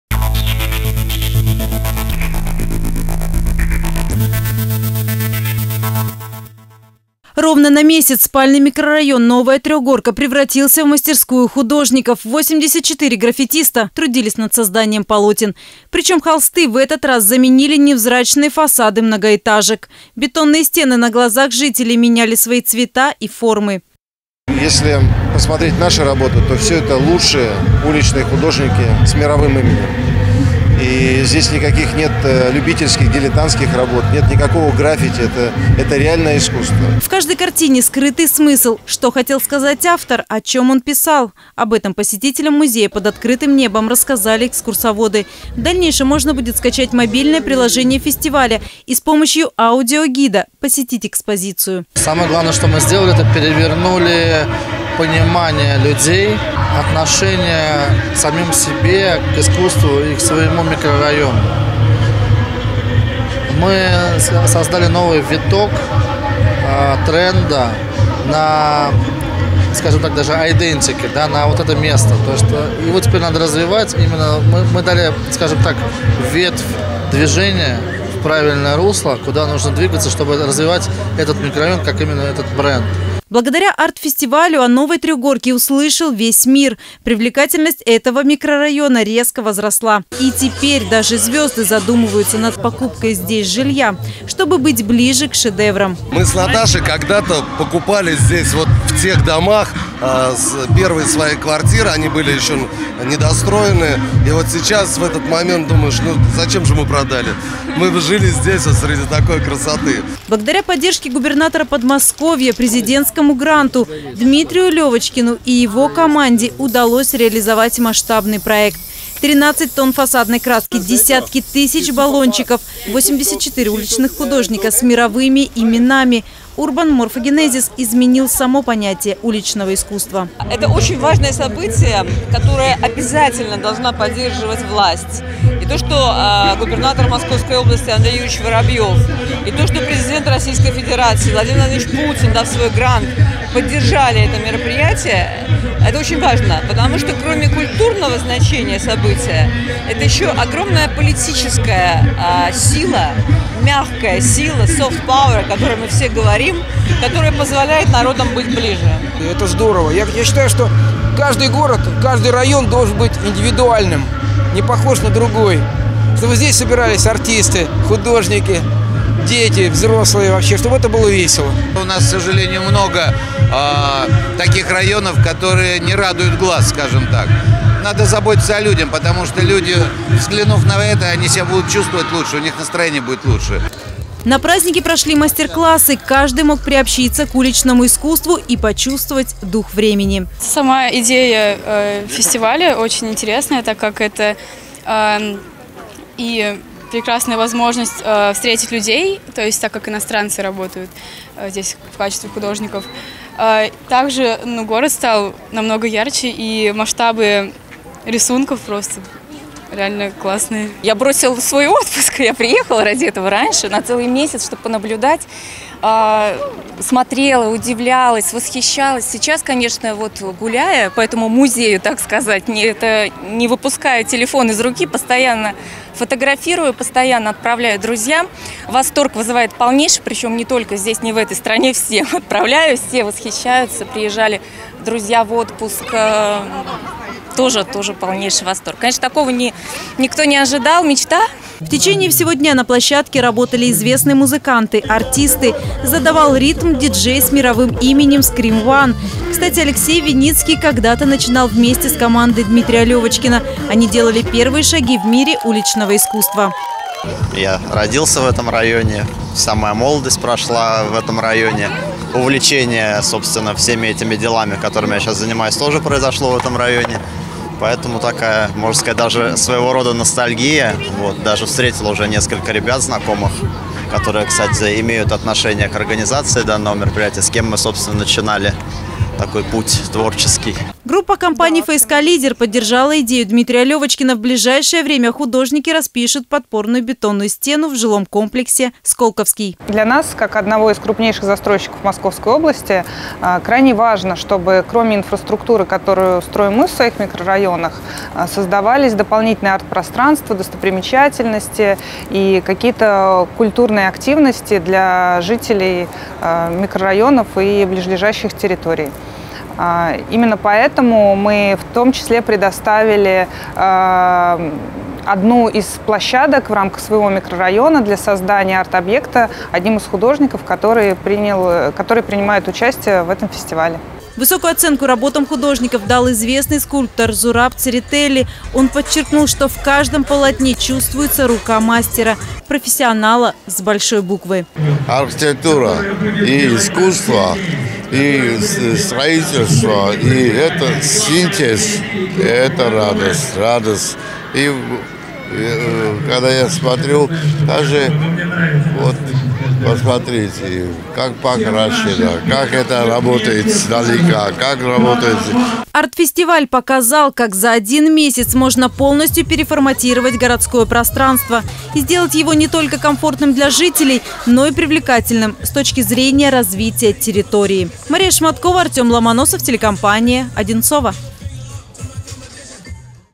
«URBAN Morphogenesis» завершился
В Одинцово завершился самый масштабный проект этого года - Арт-фестиваль «URBAN Morphogenesis» .